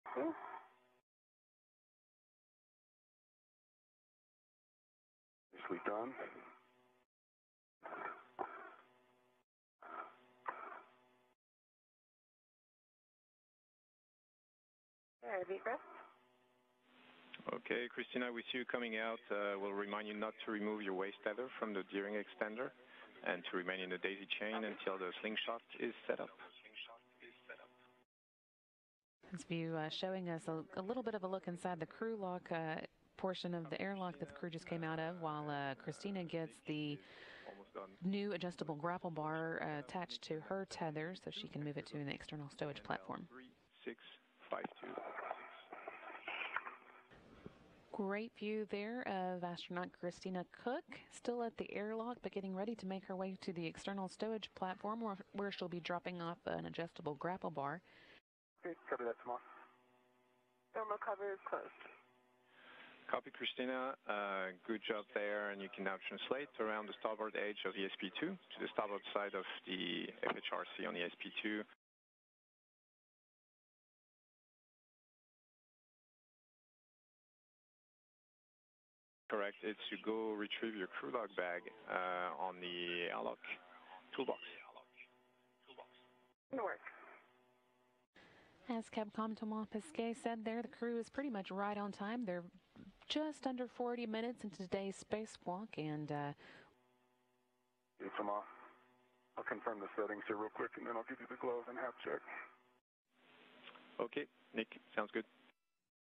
شاهد: رائدا فضاء يخرجان من محطة الفضاء الدولية في مهمة لتبديل بطاريات
قام رواد فضاء بالخروج من مركبتهم والسير خارجها للقيام ببعض الأعمال خارج محطة الفضاء الدولية.…